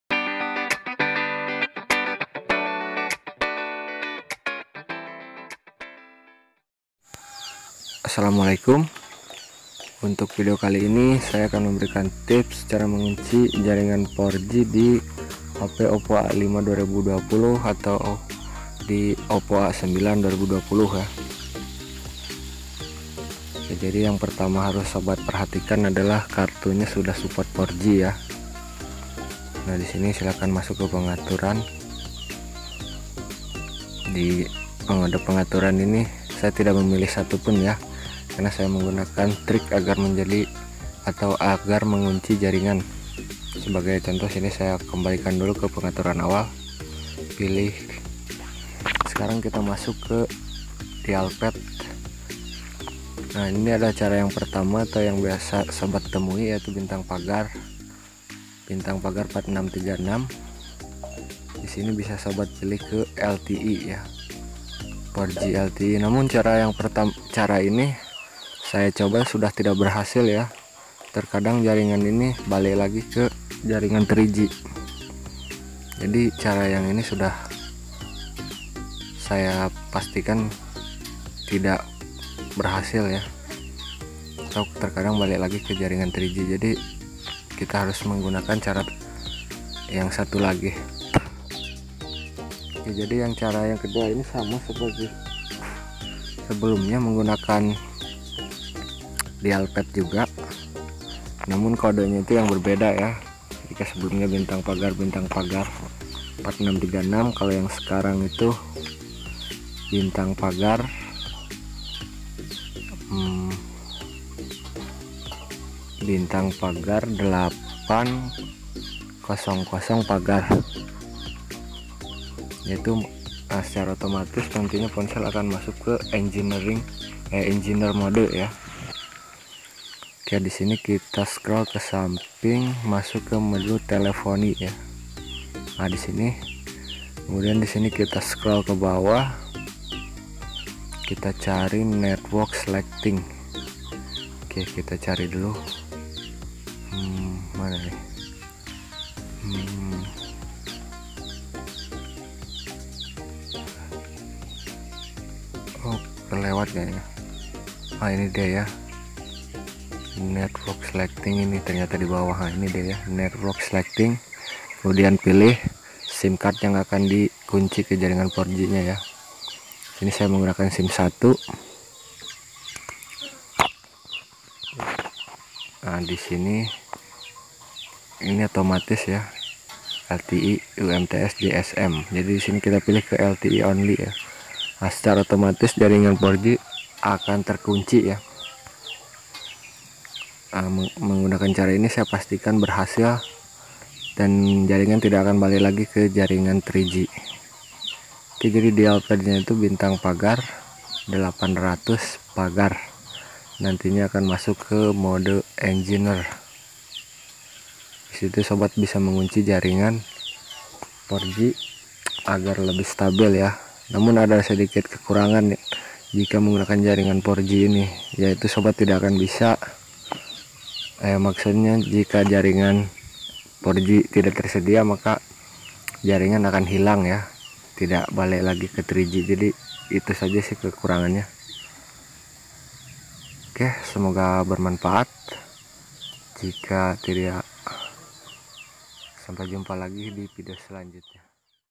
Cara Kunci Jaringan 4G OPPO A5 2020 & OPPO A9 2020
Tutorial Cara Kunci Jaringan 4G OPPO A5 2020 & OPPO A9 2020 agar jaringan lebih kuat dan stabil.